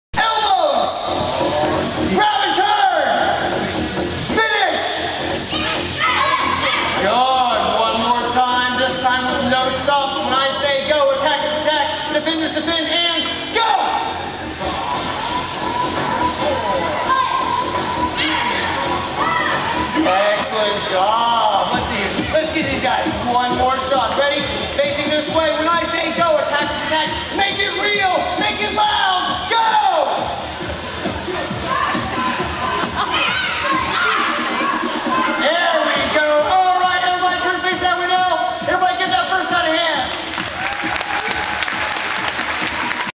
Premier Martial Arts - Graduation 12/14/13 - Amber's Black Belt Training Demonstration
Amber demonstrating her counter-attack move. Sorry for shaky camera